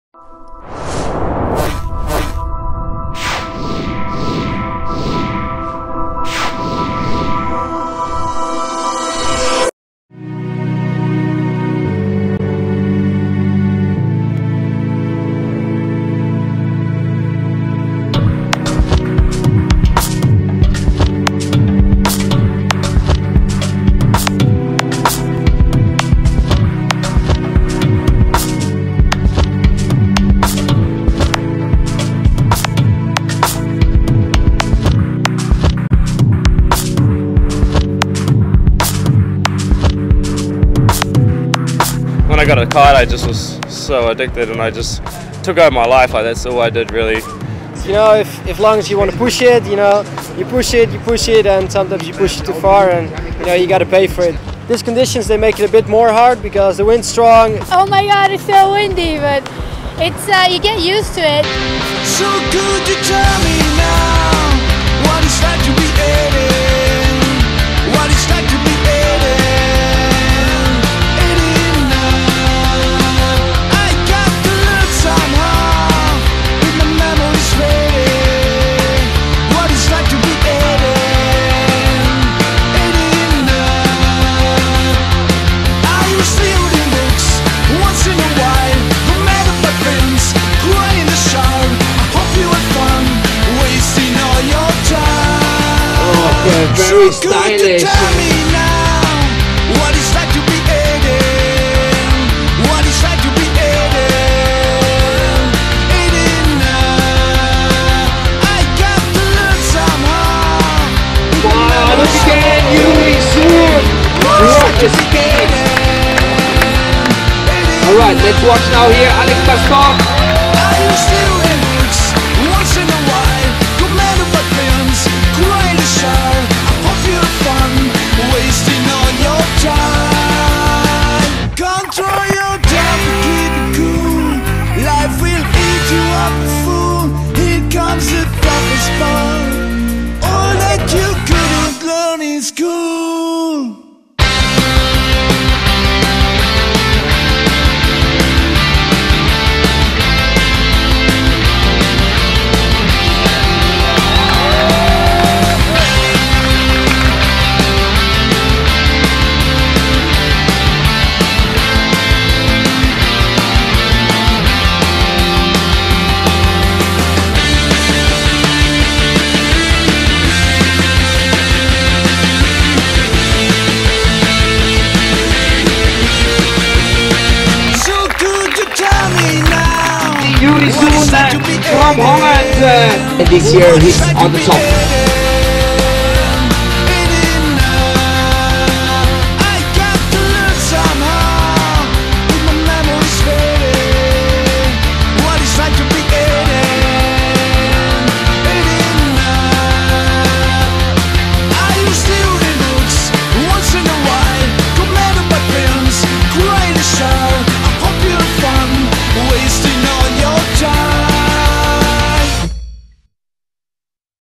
Kitesurf freestyle PKRA Fuerteventura Grand Slam 2011: Best Of !
Retrouvez toutes les plus belles images et les plus beux tricks de la compétition !